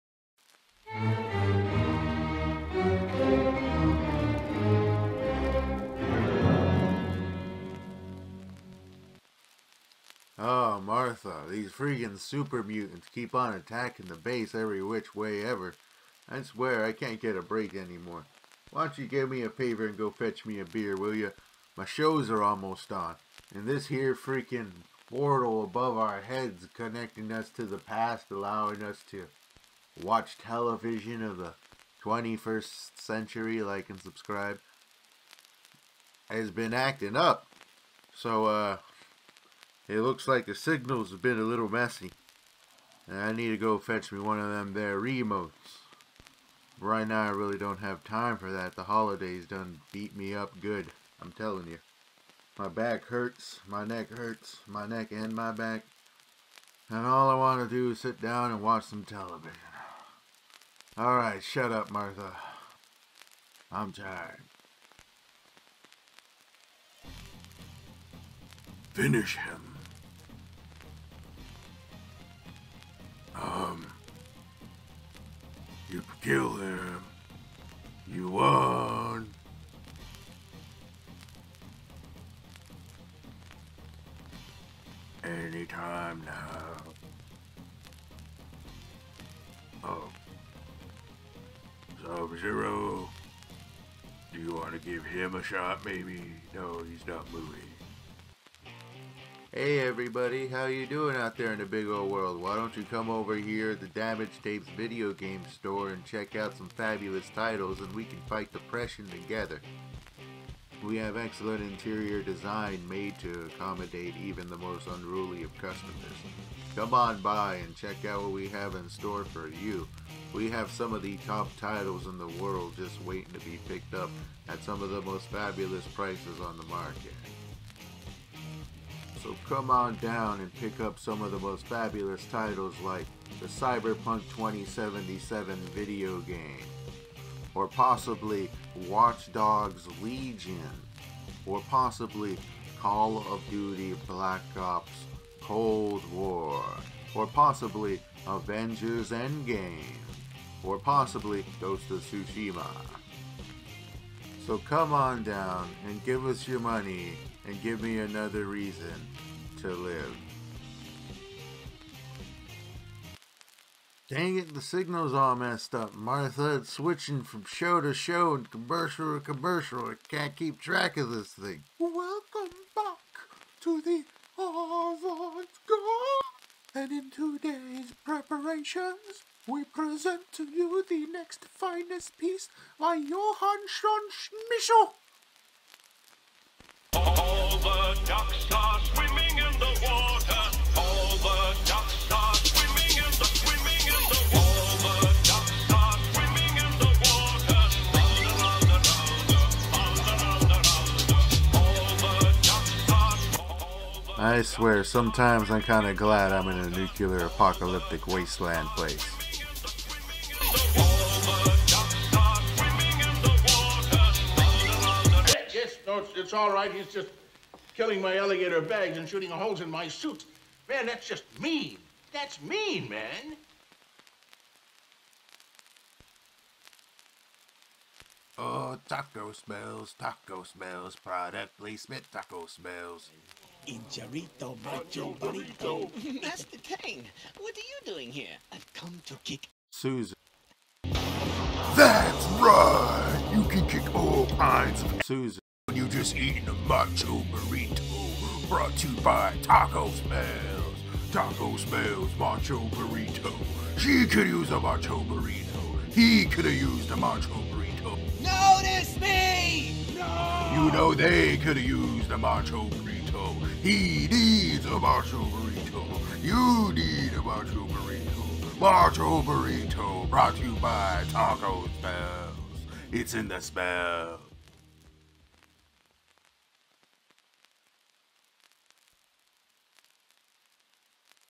Tape Damage: Broken Signal equals many skits. [Susan Edition]
The Cosmic Boyscouts TV connected to the worm hole of time is sending mixed signals to the stressed post apocalypse stay at home father.
This is our Susan Edition which means its censored...